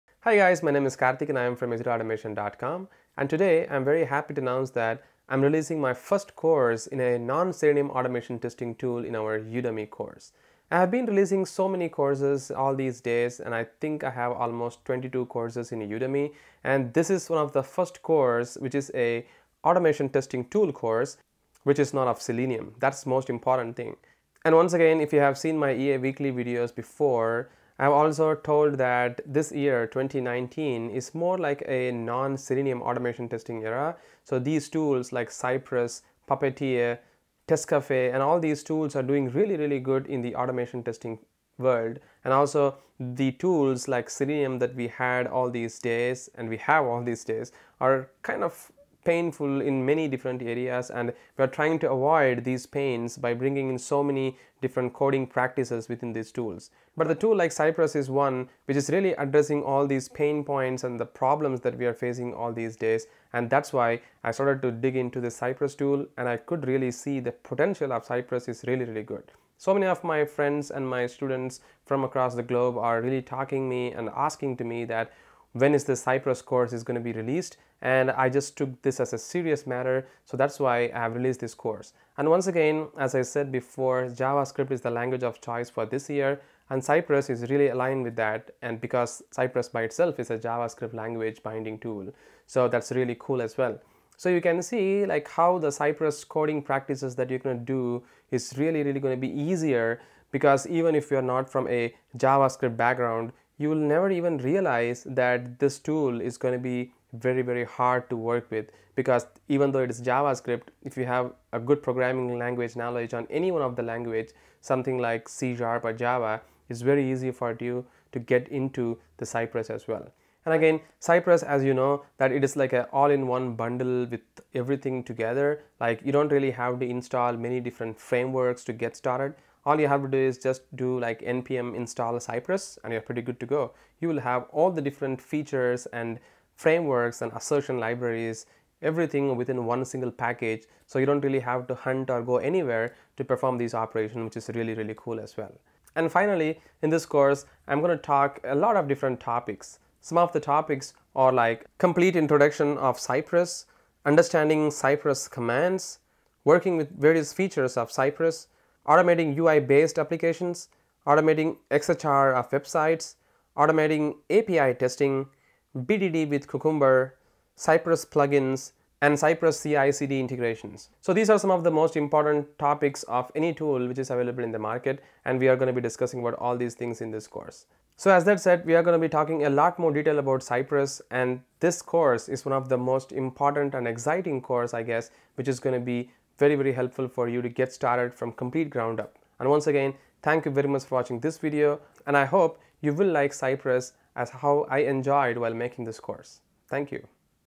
Introducing End to End automation testing with Cypress (an non-selenium testing tool)
Today I am very happy and glad to announce our first Non-Selenium automation testing tool course in Udemy on Cypress.io

Last month we discussed the Era of Non-Selenium automation testing tool and we had a really good debate on different testing tools emerging in the market.

Many community friends and people like you shown interest in Cypress and finally the course is out in Udemy now. As lots of companies are adapting Cypress in fast placid.

I am seeing a lot of excitement and enthusiasm on learning Cypress tool among the community and I hope you will all really enjoy learning the course as to how I did while making it.